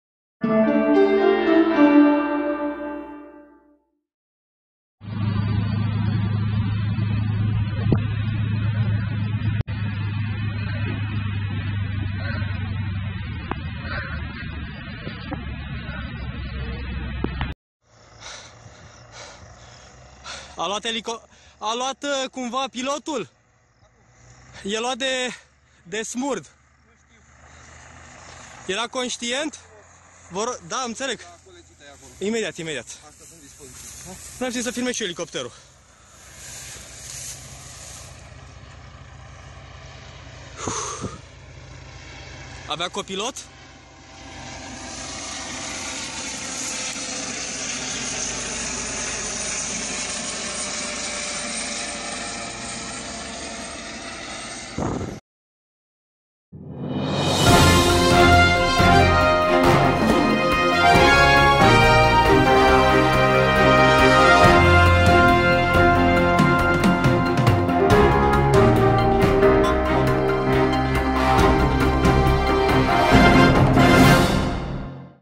ctnews.ro | MIG 21 LanceR, prăbușit la Nazarcea, Constanța
MIG 21 LanceR, prăbușit la Nazarcea, Constanța